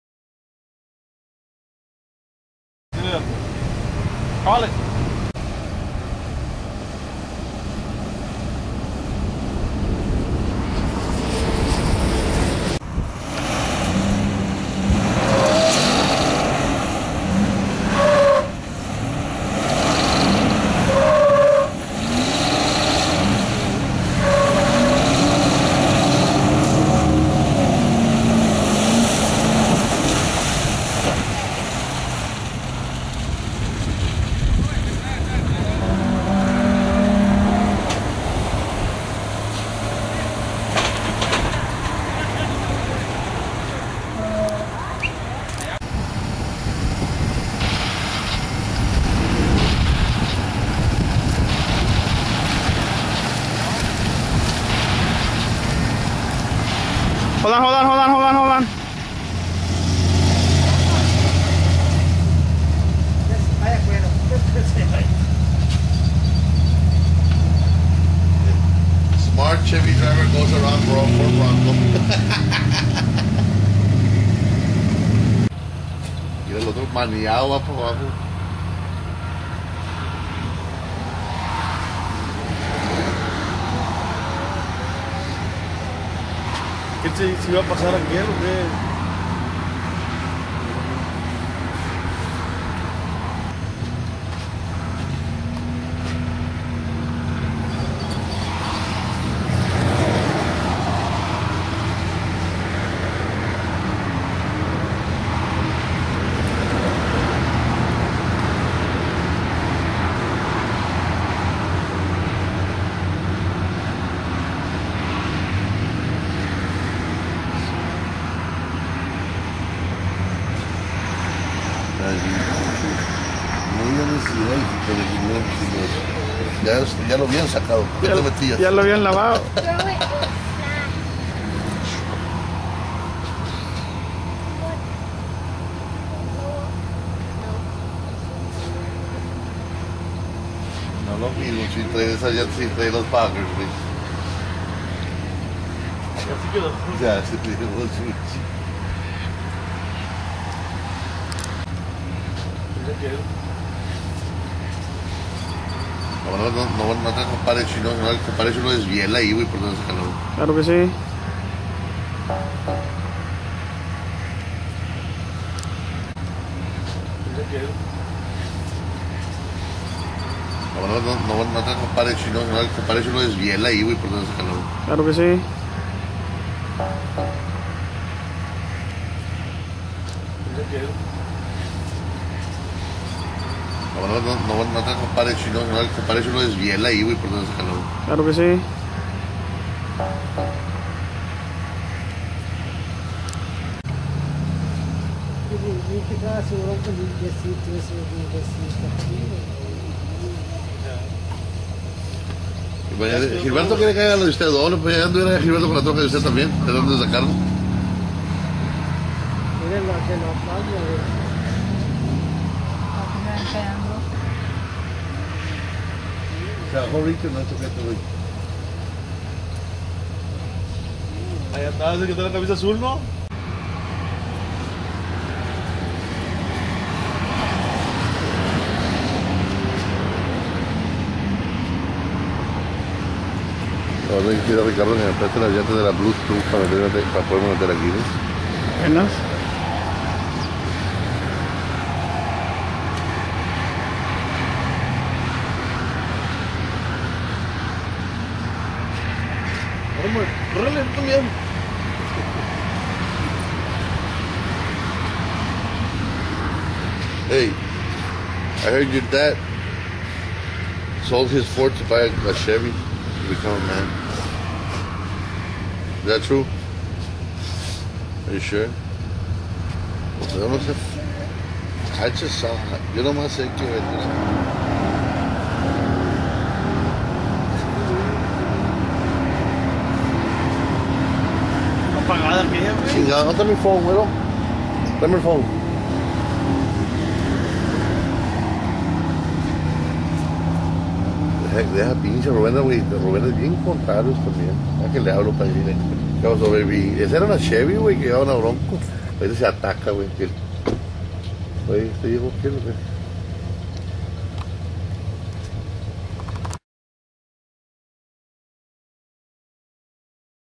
Brownsville Boat Ramp 05/18/2010
Off Roading on a Tuesday "rqtrucking" (White Bronco) goes out to play.  Quintanilla's Black Bronco & La Agujeta's Chevy join in.